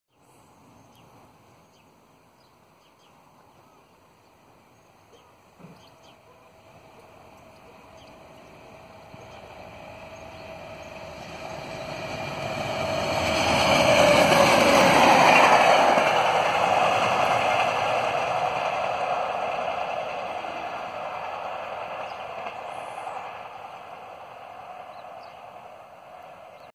Prestatyn 13.4.2018 - DRS 37425 Concrete Bob & Caroline Inspection coach
Derby to Crewe via North Wales